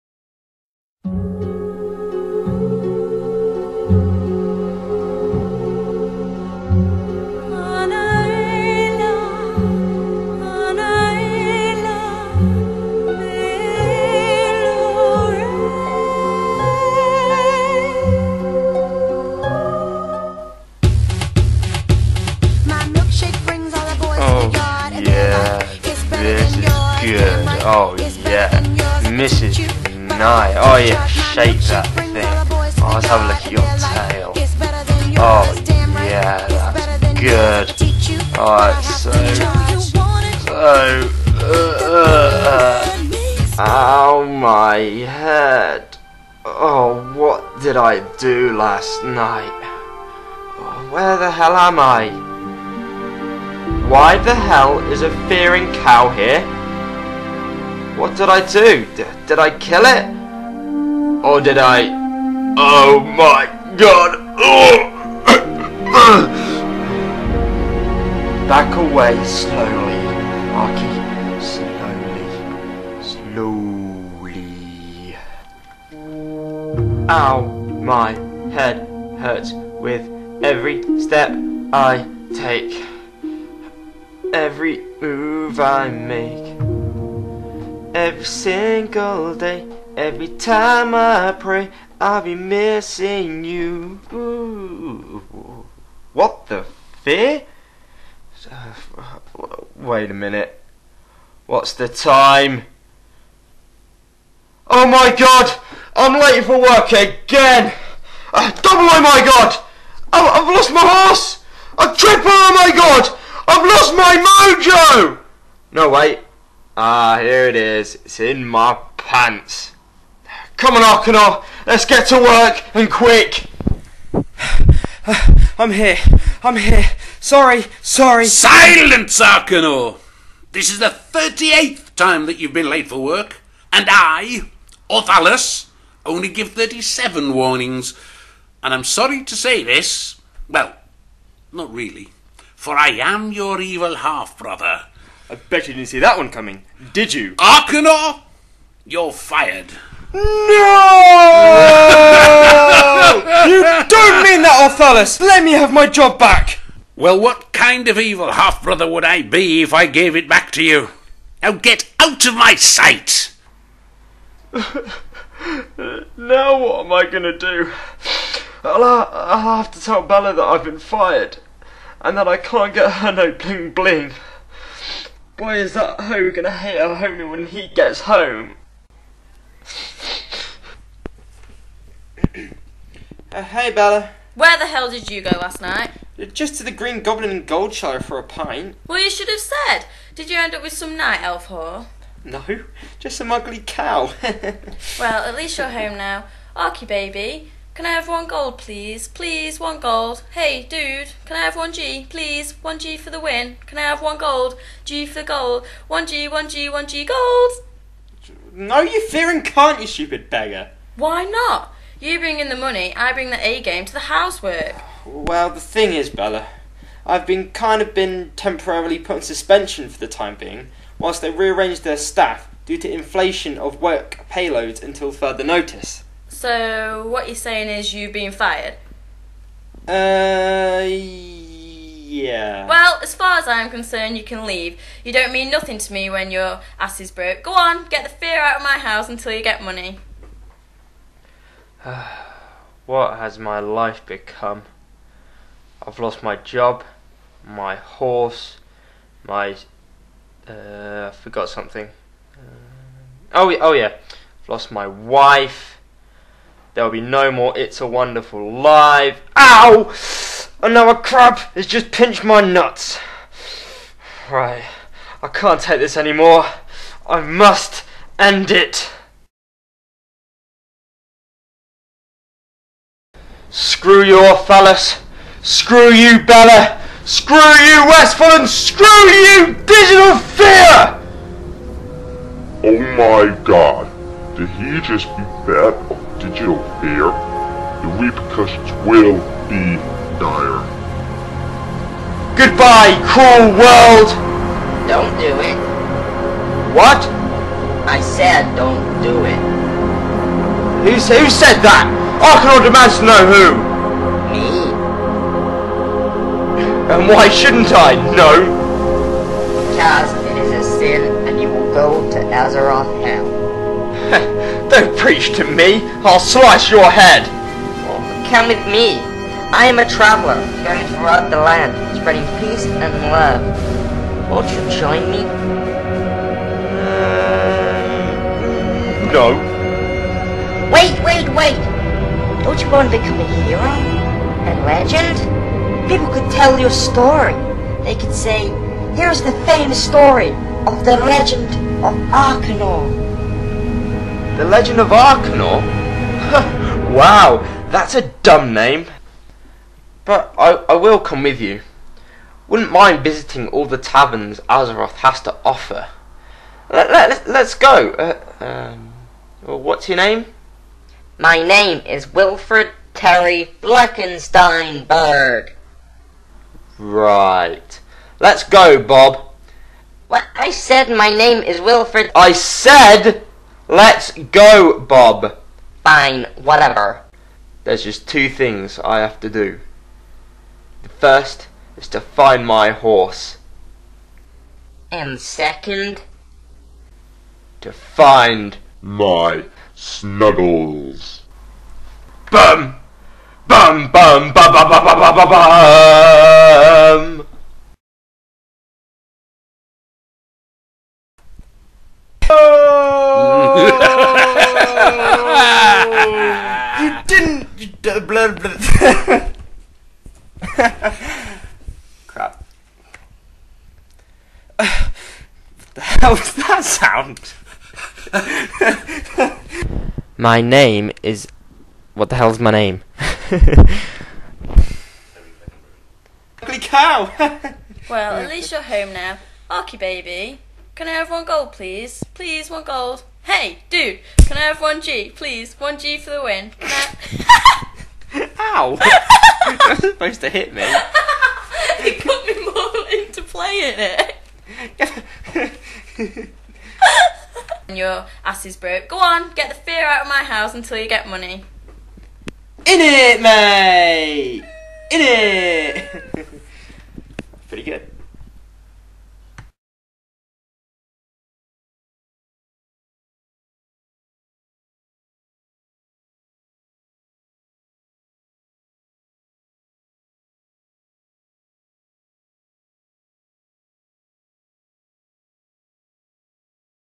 The Legend of Arcanor - Episode 2 "The End"
here it is, the end of the legend of Arcanor. or is it? You'll have to watch to find out!
Please watch Episode 1 before this if you hav'nt seen this Machinima Series before